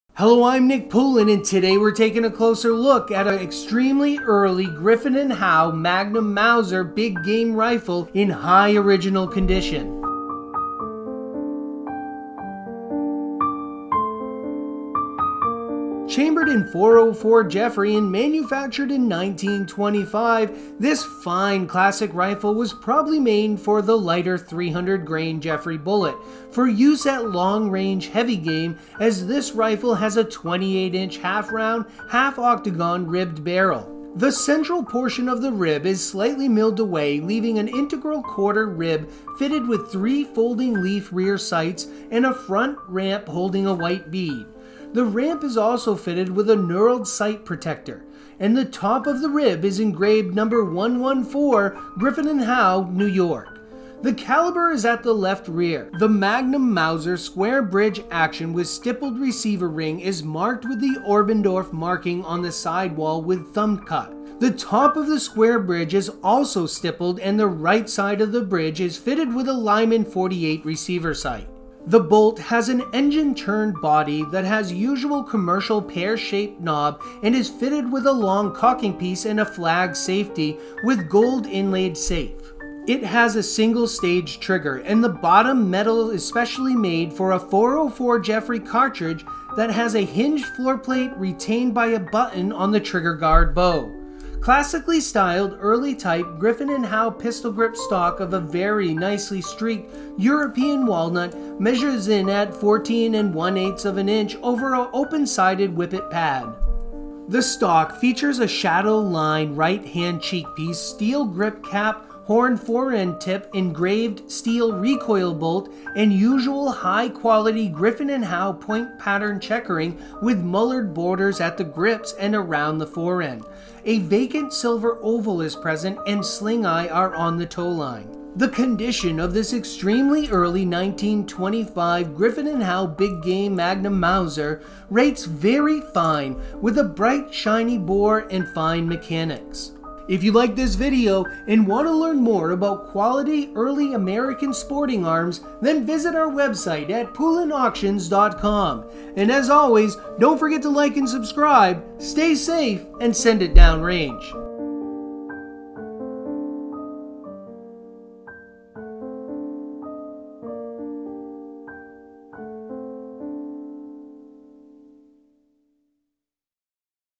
A Closer Look: Magnum Mauser Early GRIFFIN & HOWE Big Game Gun
Today we are taking A Closer Look at a SCARCE Magnum Mauser early Griffin & Howe big game gun rifle in HIGH ORIGINAL condition, chambered in 404 Jeffery!

00:00 Intro
00:16 Caliber, Barrel, Sights
00:56 Action & Receiver
01:23 Trigger & Hinged Floorplate
01:34 Stock
02:08 Condition

For more information on this incredible Mauser rifle, or to view additional collectible and historic firearms and military antiques, please visit our